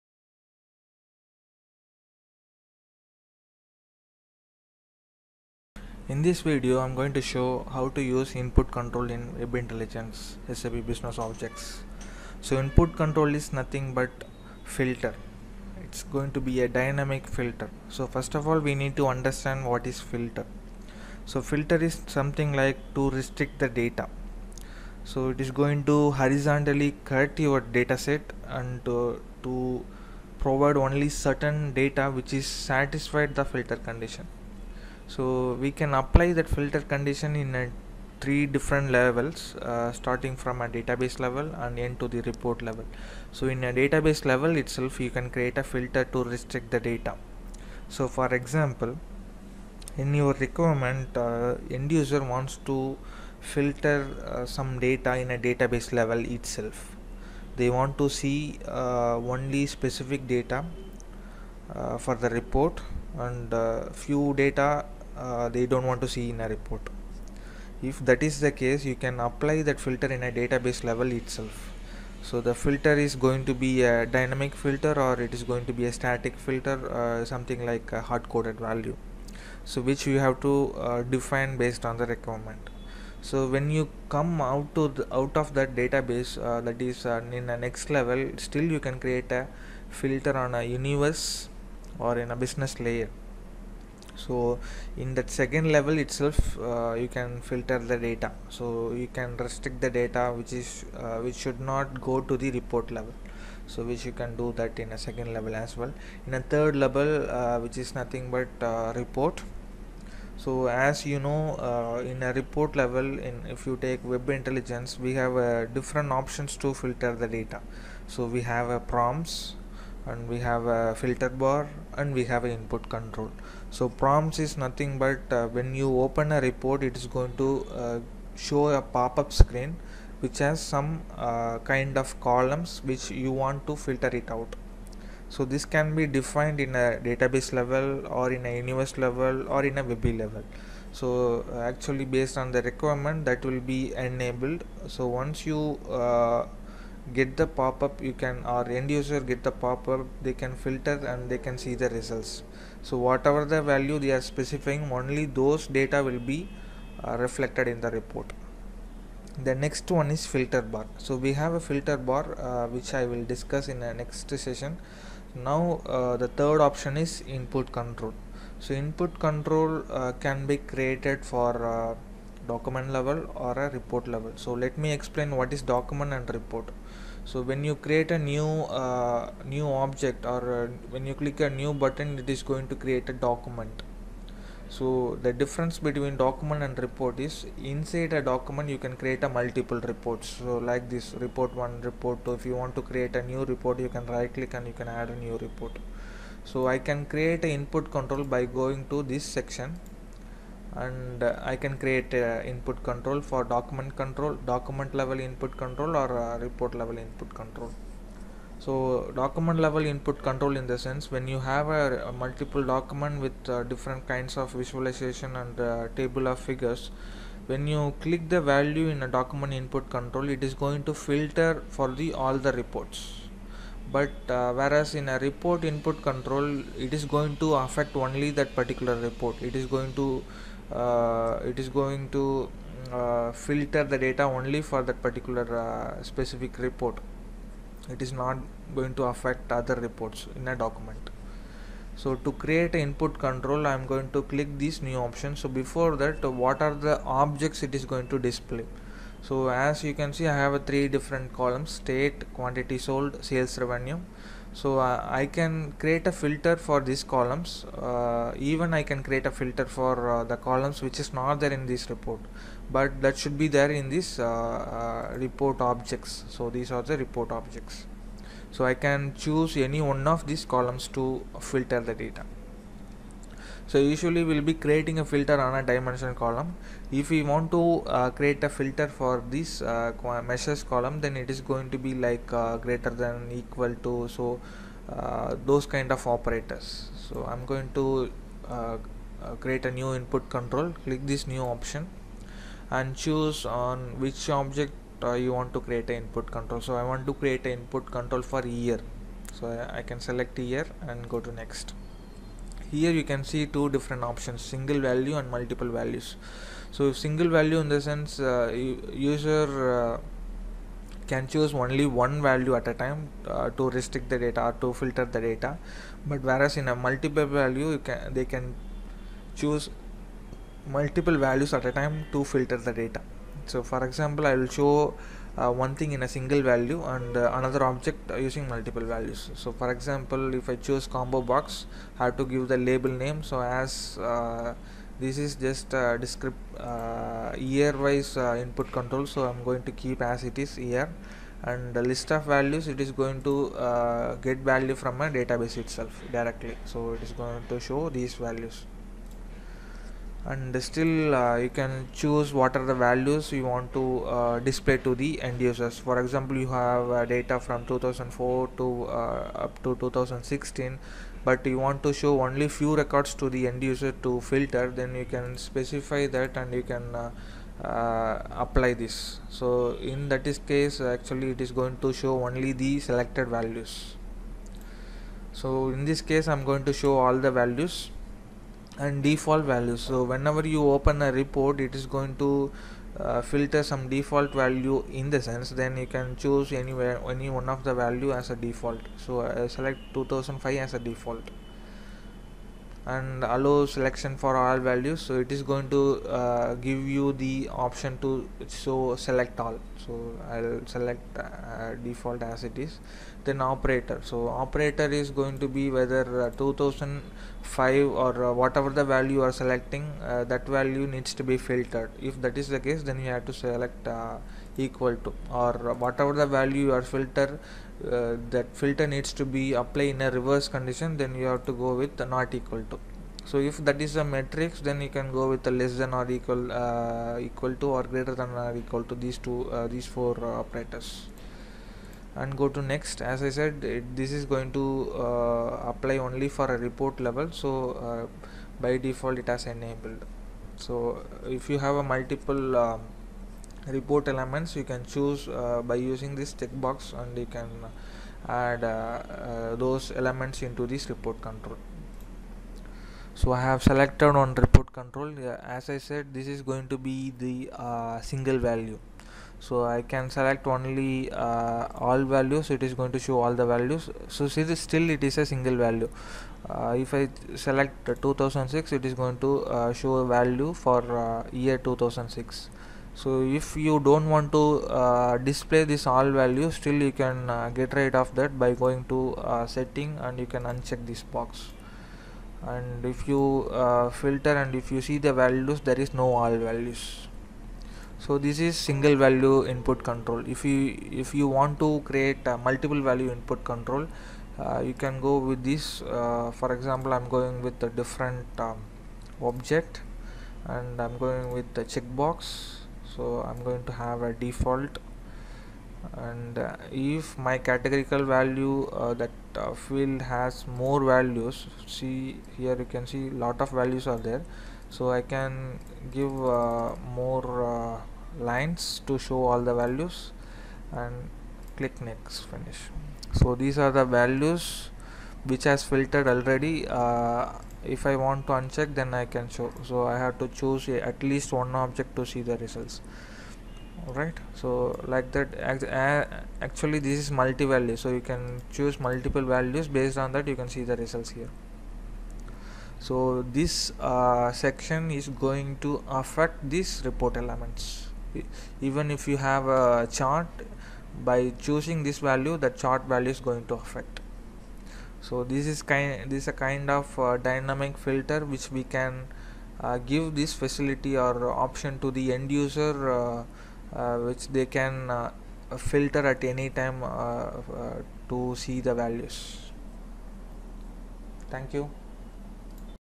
Input Control in Web Intelligence - Real Time Scenario
Understand the Filter in End to End scenario and specifically Input control